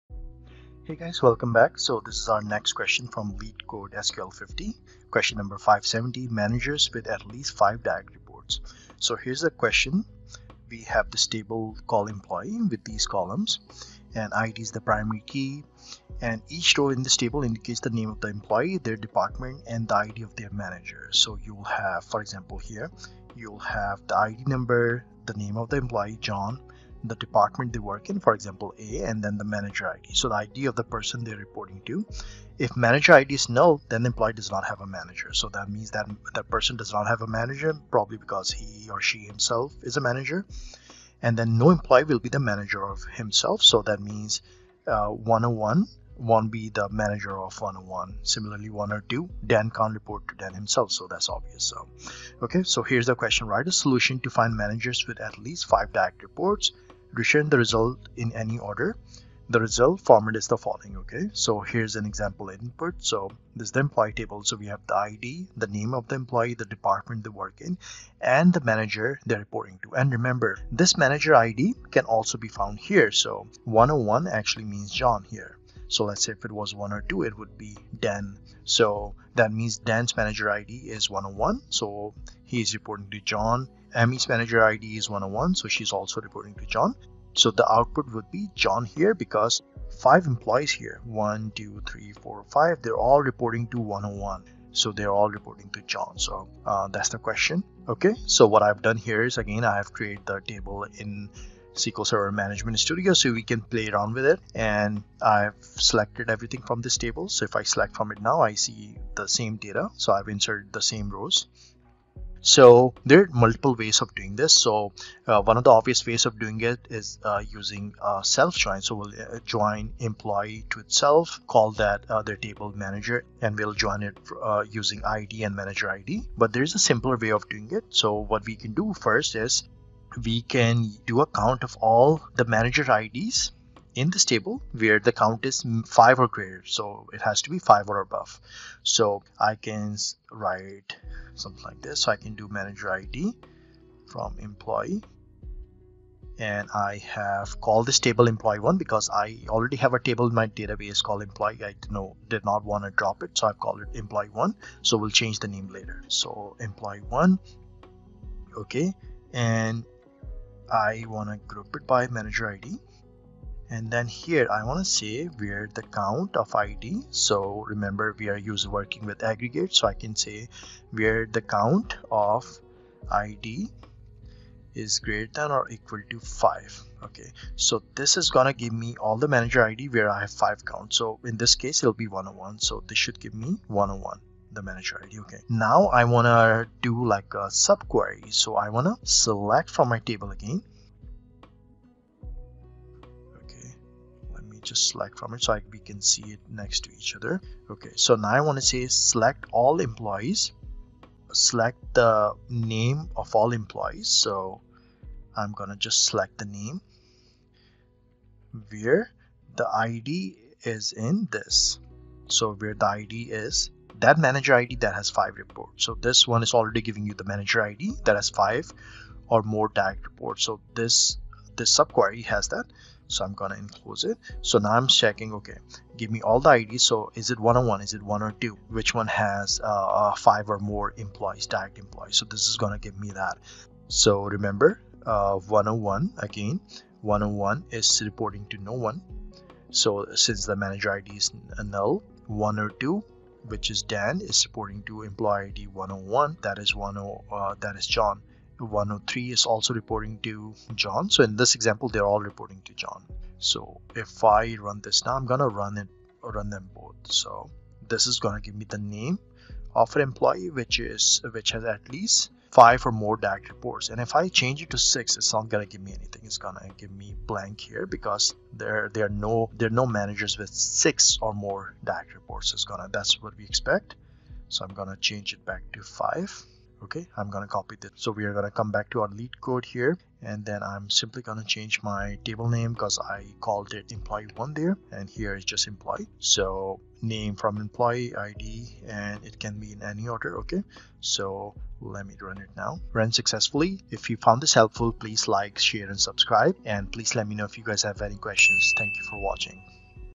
SQL Leetcode SQL 50 #13: Managers with at Least 5 Direct Reports.
SQL Leetcode SQL 50 #13: 570: Write a solution to find managers with at least five direct reports. We will be solving all the questions from LeetCode SQL 50 one by one. These questions range from easy to hard.